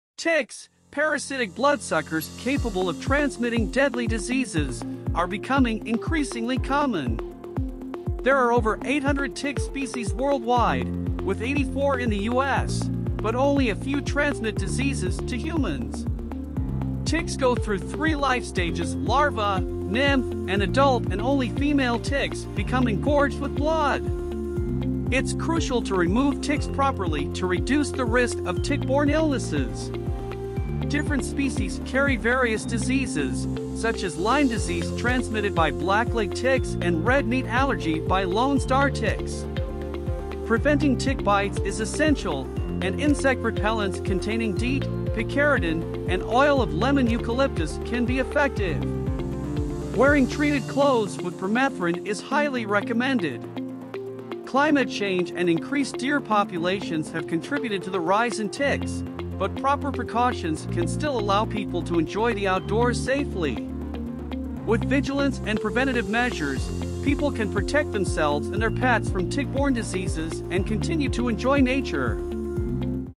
Tick Awareness Protecting Your Health Outdoors.
Ticks, parasitic bloodsuckers capable of transmitting deadly diseases, are becoming increasingly common. There are over 800 tick species worldwide, with 84 in the US, but only a few transmit diseases to humans. Ticks go through three life stages - larva, nymph, and adult - and only female ticks become engorged with blood. It's crucial to remove ticks properly to reduce the risk of tick-borne illnesses. Different species carry various diseases, such as Lyme disease transmitted by blacklegged ticks and red meat allergy by lone star ticks. Preventing tick bites is essential, and insect repellents containing DEET, picaridin, and oil of lemon eucalyptus can be effective. Wearing treated clothes with permethrin is highly recommended. Climate change and increased deer populations have contributed to the rise in ticks, but proper precautions can still allow people to enjoy the outdoors safely. With vigilance and preventative measures, people can protect themselves and their pets from tick-borne diseases and continue to enjoy nature.